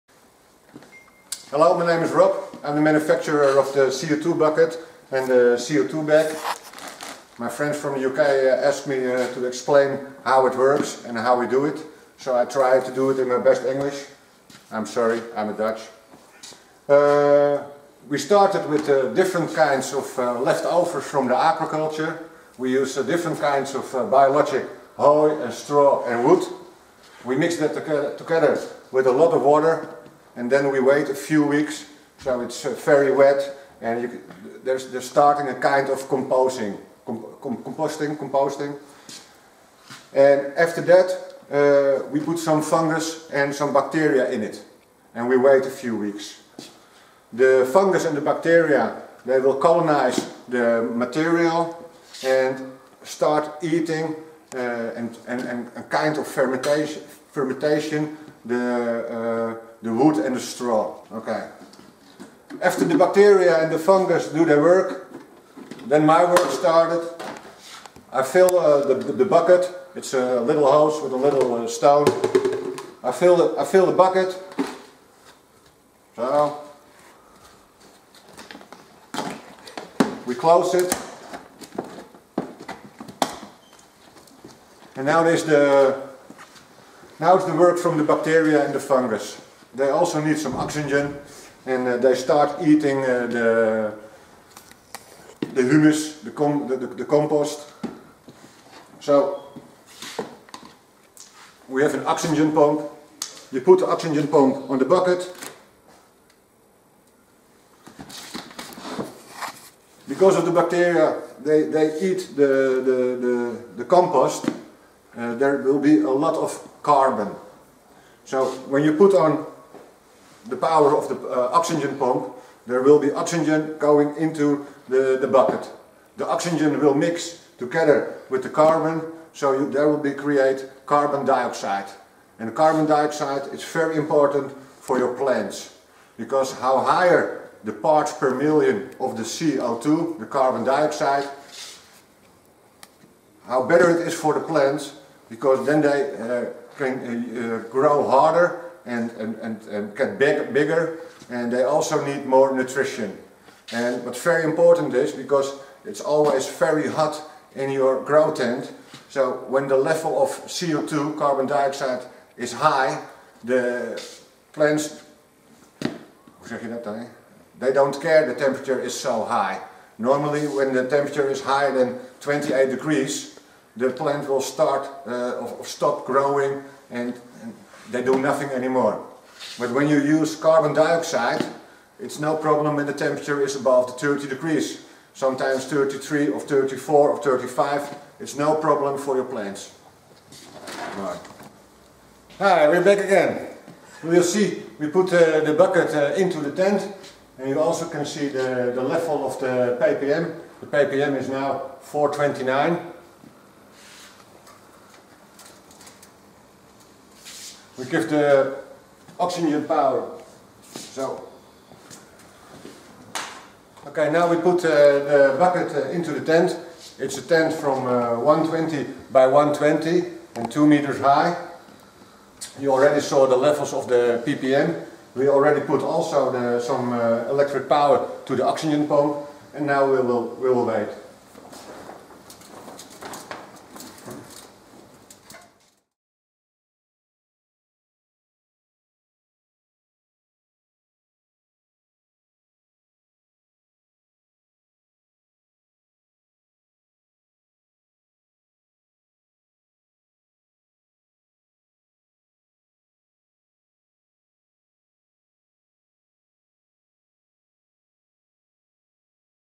SeeSoGreen CO2 Generator Bucket
Introducing Seesogreen - The organic co2 generator
The Seesogreen Organic CO2 Generator is created of 100% organic raw material that doubles the amount of CO2 in your grow room up to 12 hours a day. The doubling of CO2 is created by 100% organic raw materials in combination with inserted CO2. The silent air pump is included and simple to install.
What is the Seesogreen CO2 Bucket?
Through its unique formula (that works!!) of organic bio-mass and expanded bacteria, you can always be confident of a high CO2 level +/- 700ppm. We work exclusively with organic waste from the agricultural sector; wood, straw, hay and various grains. All this top quality sourcing and research results in a fantastic product!
In Short, Seesogreen CO2 Bucket.
• Creates CO2 to get a higher CO2 level.
• CO2 will stimulate and activate to absorb more nutrients.
• CO2 will give you higher results.
• Contains 100% organic materials
• Tested formula
What is CO2?
CO2 is a molecule of carbon (C) and a molecular oxygen (O2). By putting the bacteria into the bio-mass it will create a colony of bacteria. These bacteria need food and will eat the bio-mass, this creates carbon (C). Because there is oxygen (CO2) added in the Seesogreen CO2 Bucket through the pump, the oxygen (CO2) will bind to the carbon (C) and there will be CO2 created.
Why CO2?
CO2 is a building block, think of CO2 as a kind of cement. That cement is needed as a chemical reaction of photosynthesis (assimilation). Plants convert light into energy. The higher the CO2 level the better the photosynthesis. Crops take through their stomata CO2 and separate the CO2 in carbon (that will be absorbed) and oxygen (O2) will be left through the stomata of the crops again. The crops are able to absorb more nutrients, which results in higher yield of the crops. By a higher CO2-level (ppm +/- 600-max 800), the plants are more resistant to higher temperatures (+/- 32 degrees Celsius).
The average CO2 outside is +/- 300-400ppm, on the edge of the city is the CO2 slightly higher (PPM = parts per million). Based on the average ppm (+/- 300-400ppm), therefore its advisable to use Seesogreen CO2 Bucket to double your ppm, helping the plants absorb more nutrients, at higher temperatures resulting in higher yields.
When to use the Seesogreen CO2 Bucket or Seesogreen CO2 Bag
The Seesogreen CO2 Bucket or Seesogreen CO2 Bucket 10 litre Bag should be used in the 3rd week of flowering. Using CO2 before this time is inefficient because the crops are still full of growth hormones, and chances of plant stretching could happen. It is also important that the CO2 is used at the time the lighting (12 hours) is turned on and then stops when the lights turned off (go out).
More CO2 (ppm) in combination with light (energy) leading to increased yields.
Why the Seesogreen CO2 Bucket?
The Seesogreen CO2 Bucket has been developed to create CO2 so that the grow crops are able to absor more nutrients and will give the best end results. Through the unique (tested) composition of our organic biomass and expanded bacteria, you can always be sure of a high CO2 level +/- 700ppm (ppm=parts per million).
Who is behind the Seesogreen CO2 Bucket
The Seesogreen CO2 Bucket is developed by a dutch company, that specialises in organic formulas and advises commerical farming companies world wide. The materials that they provide are fully organic materials, which give a 100% organic end product.Love it, have to have it?